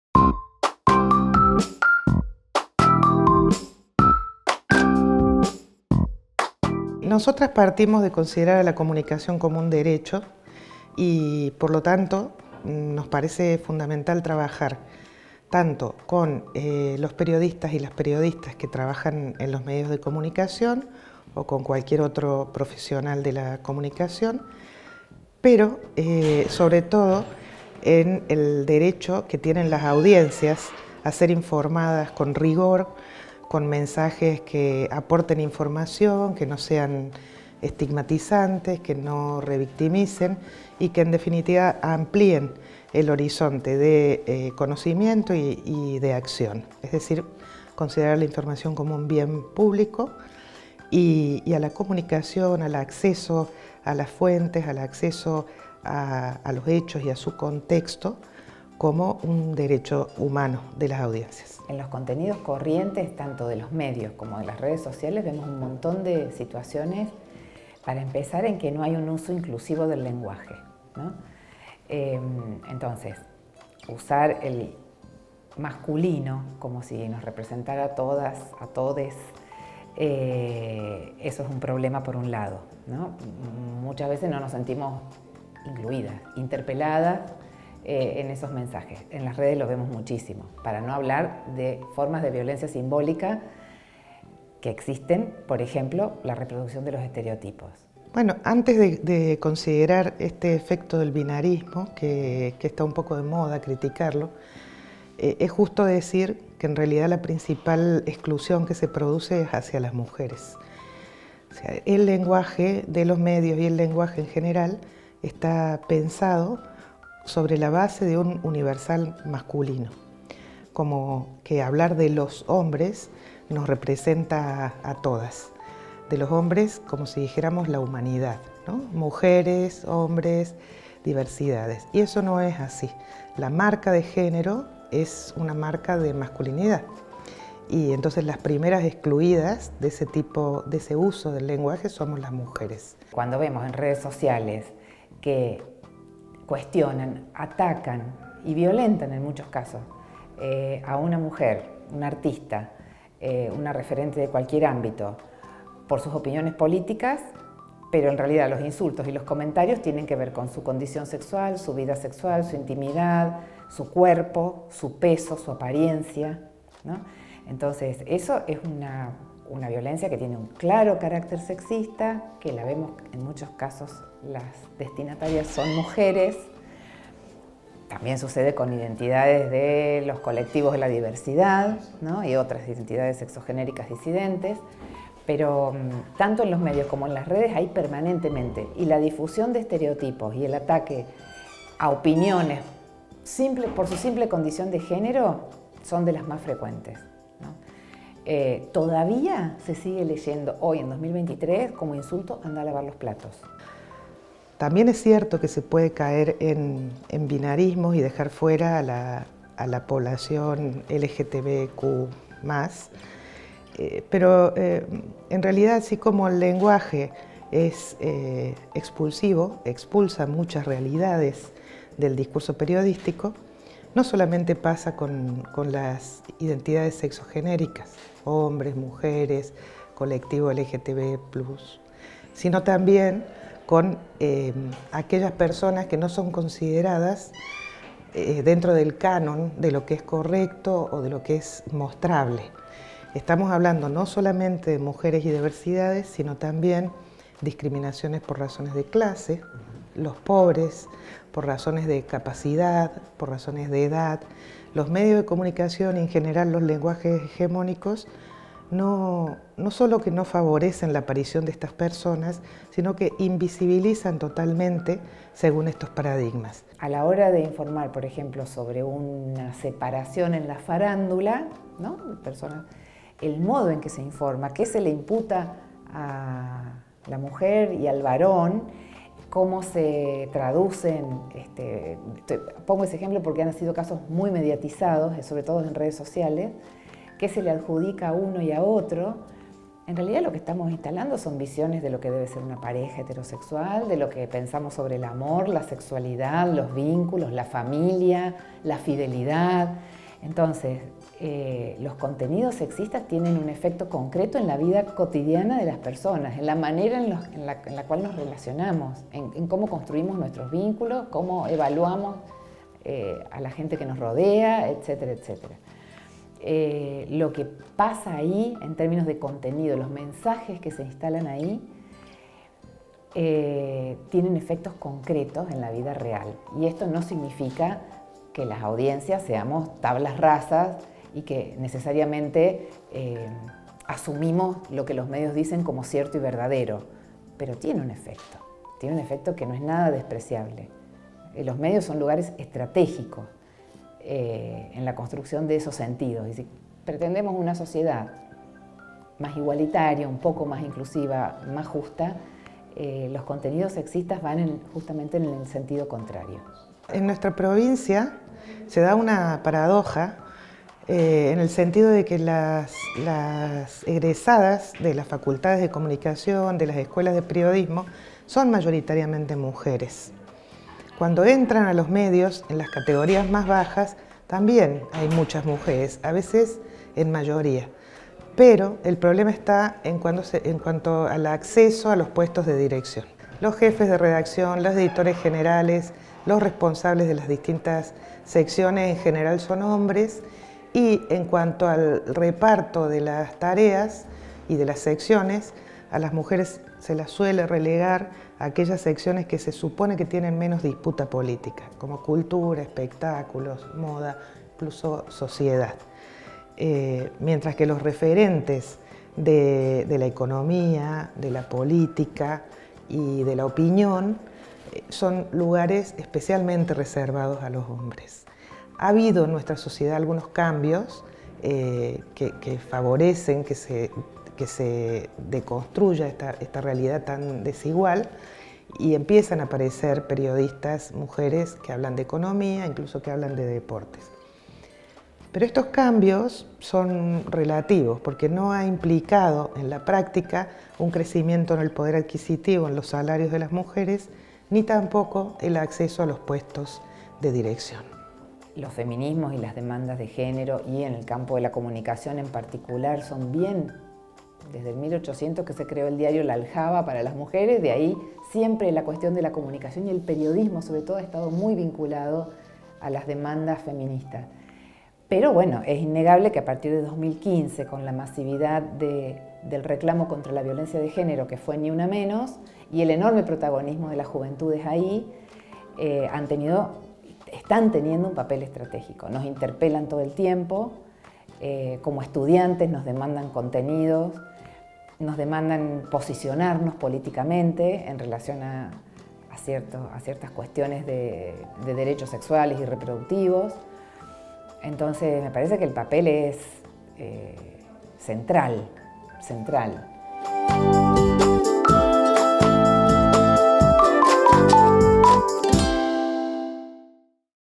UNIJOVEN - COMUNICACIÓN NO SEXISTA
Dos periodistas de la Red Par nos invitan a pensar el lenguaje como una herramienta para visibilizar e incluir identidades de género. También reflexionar sobre la responsabilidad de los medios para comunicar, sin discriminar...
UniRio TV
Universidad Nacional de Río Cuarto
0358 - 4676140